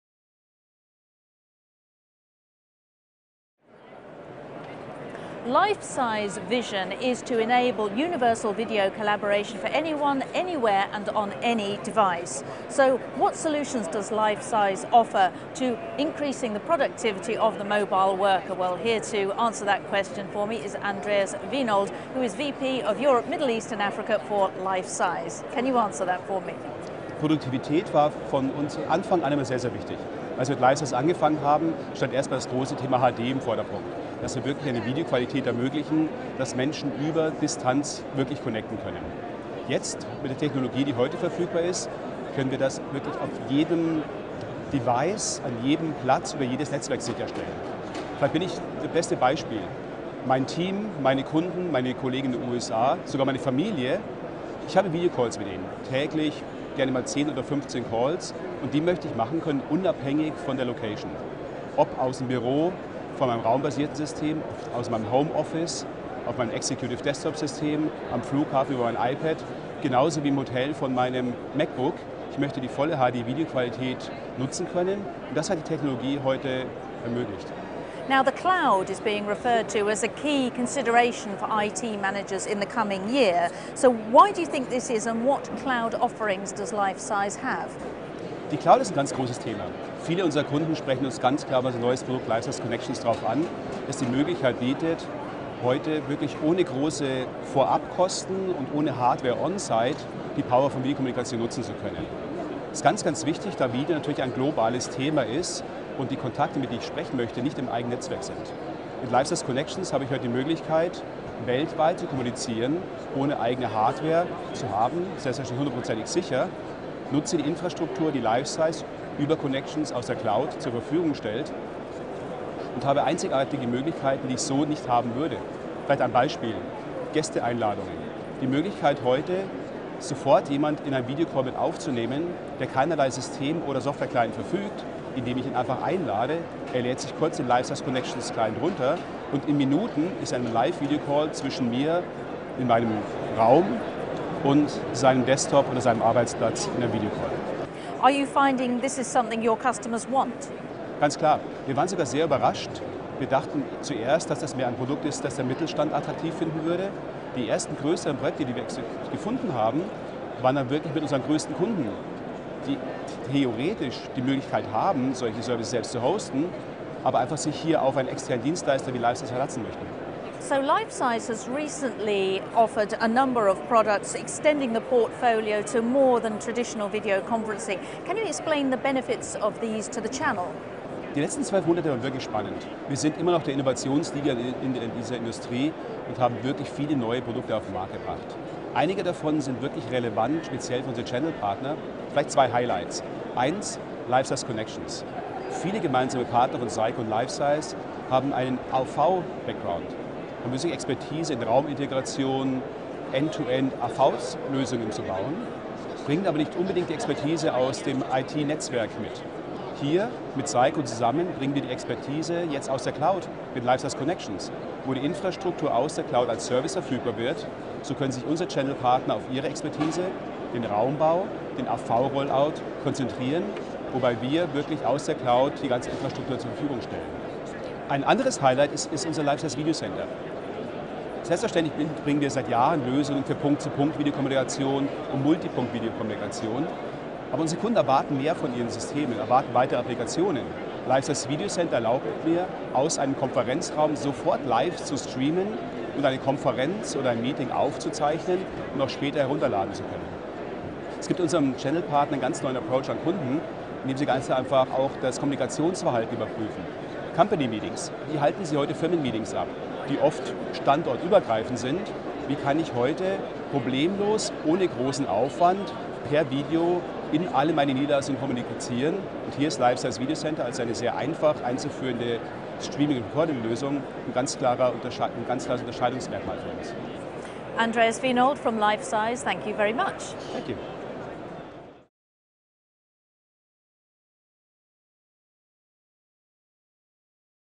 LifeSize - für jedermann, an jedem Ort und mit jedem Device
Andreas Wienhold, LifeSize Vice President Sales EMEA, spricht mit Zycko TV über Videokonferenzlösungen für jedermann, an jedem Ort und mit jedem Device.